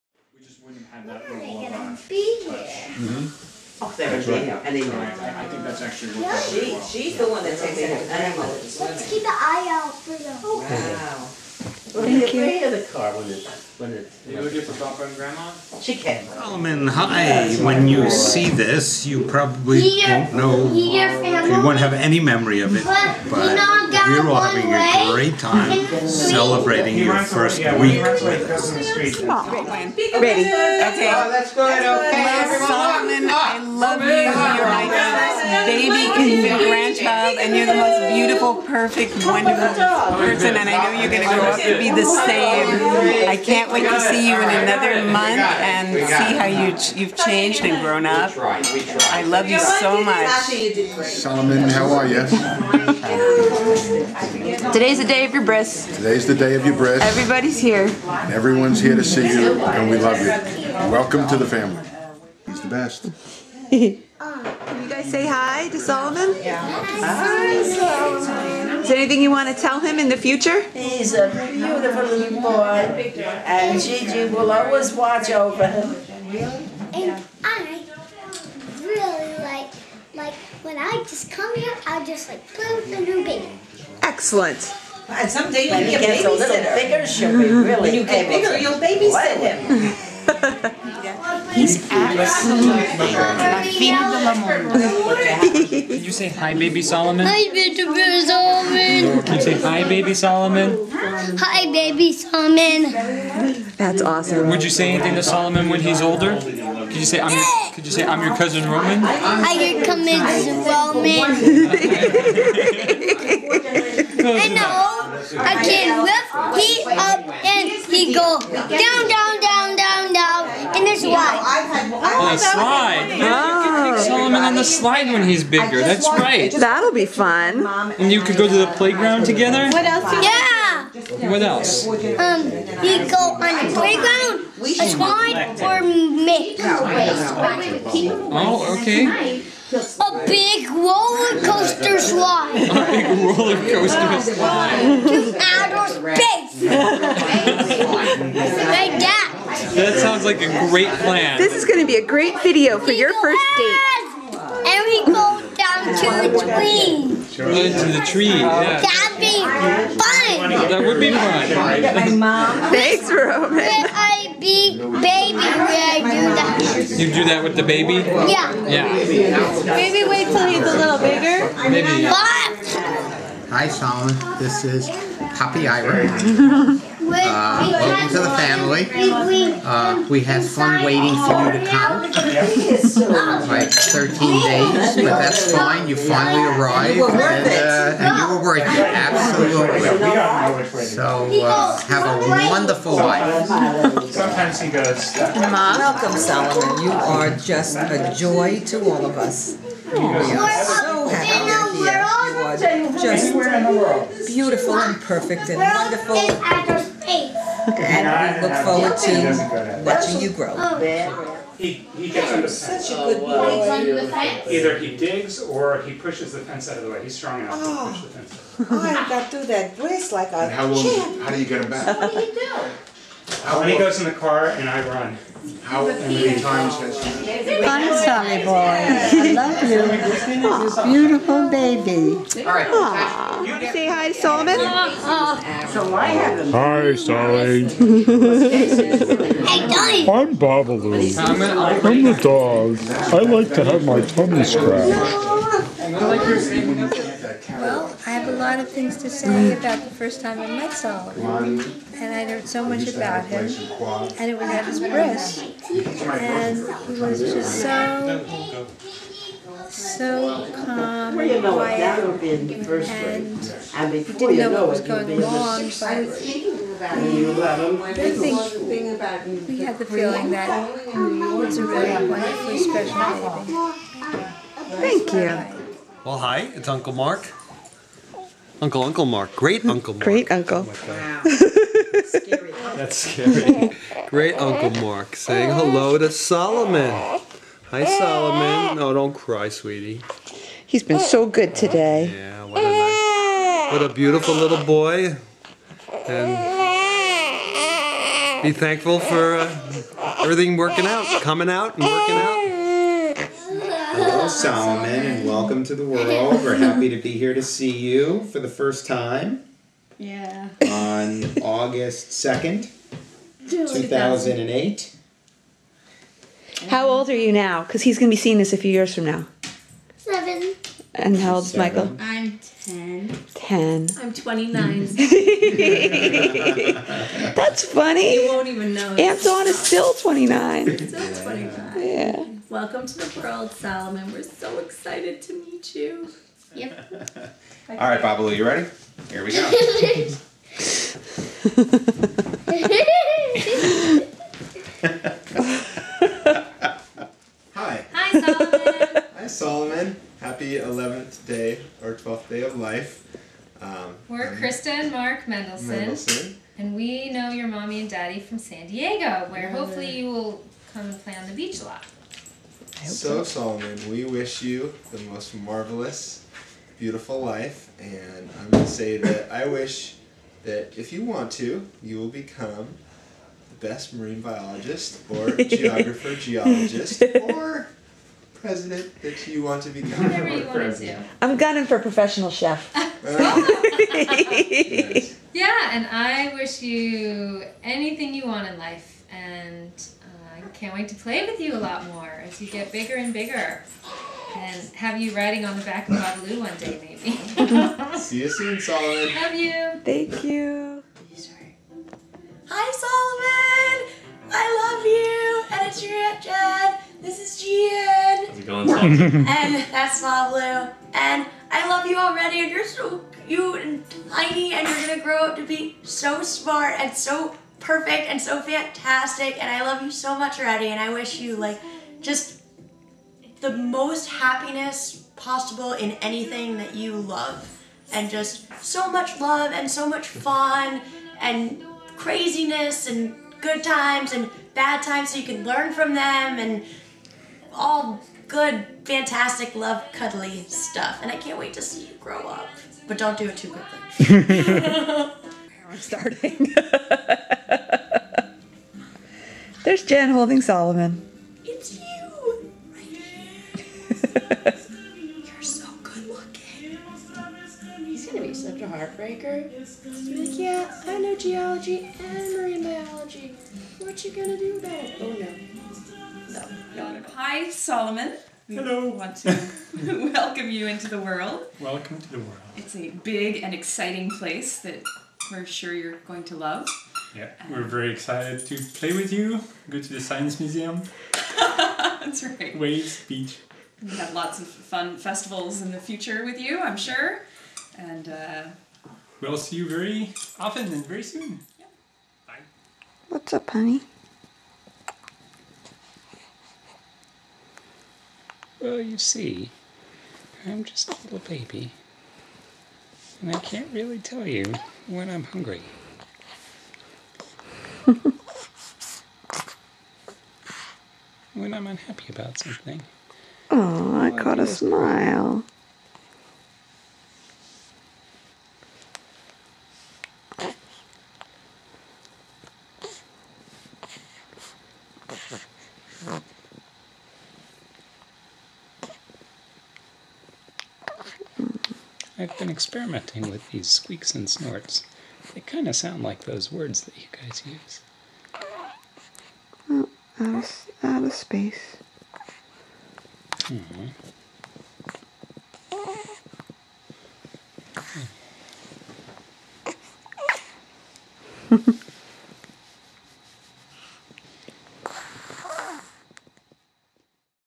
Welcome Solly Volume 1
Video of friends and family welcoming Solly into the world.